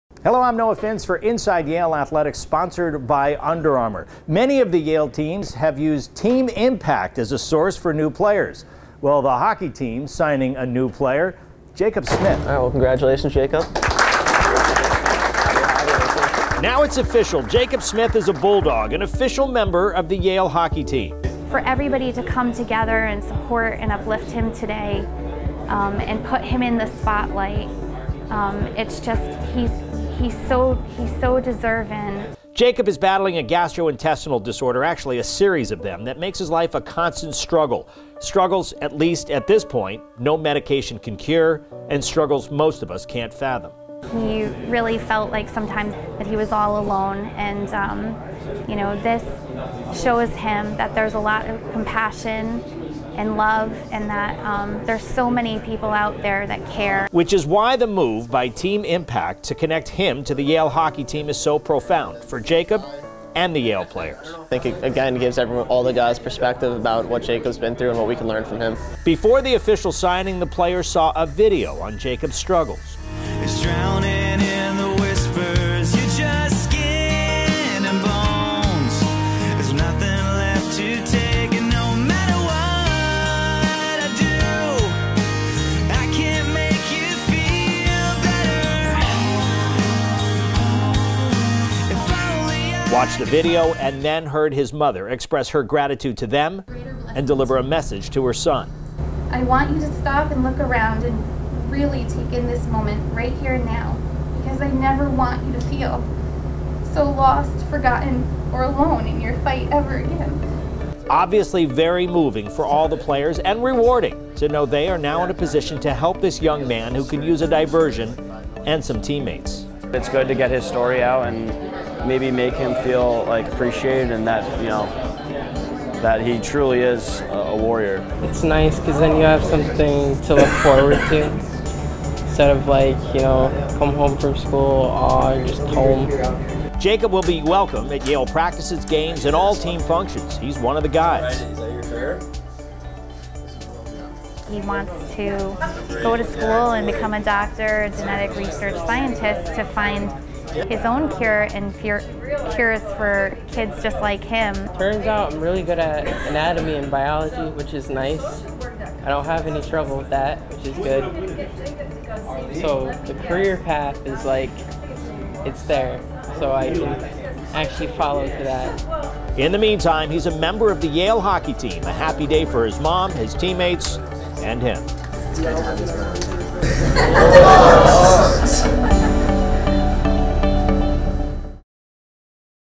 Inside Yale Athletics Sponsored by Under Armour: Men's Ice Hockey and Team IMPACT
The Yale men's ice hockey team "drafts" a new player through Team IMPACT.